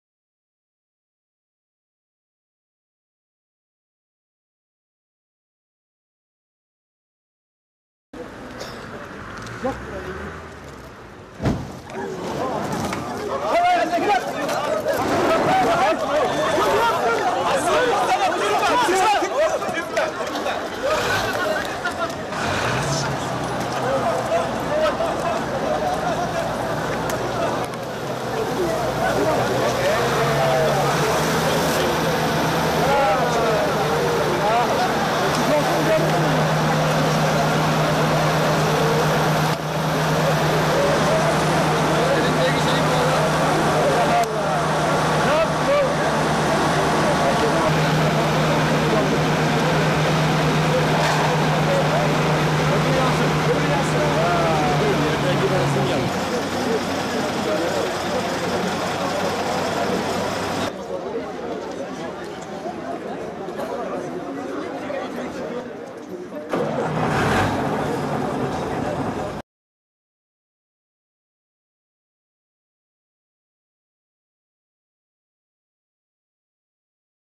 Can Pulak Bodrum gündem Yazıları - YANGINI BENZİNLE SÖNDÜRMEK
Her kafadan bir sesin çıktığı, milli disiplinin dağıldığı ve siyaset dilinin kızıştığı bir ortamda ülkemizi aydınlık bir geleceğe taşıyamaz, dağ gibi biriken çok ciddi sorunların altından kalkamayız...